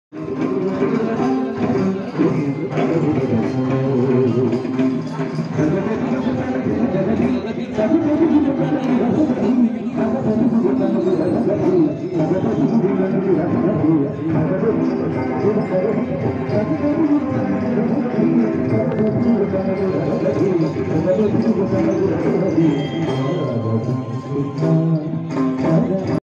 Deepakazhcha Nooranad Padanilam Parabrahma Temple 25/11/2022 പടനിലം പരബ്രഹ്മ ക്ഷേത്രം-ദീപക്കാഴ്ച 2
Deepakazhcha Nooranad Padanilam Parabrahma Temple  25/11/2022  പടനിലം പരബ്രഹ്മ ക്ഷേത്രം - ദീപക്കാഴ്ച

Deepa kazhcha  @ Nooranad Padanilam Parabrahma Temple  25/11/2022
നൂറനാട് പടനിലം പരബ്രഹ്മ ക്ഷേത്രത്തിലെ 2022 ലെ വൃശ്ചിക മഹോത്സവത്തോടനുബന്ധിച്ച് നടന്ന 25/11/2022 ലെ ദീപക്കാഴ്ച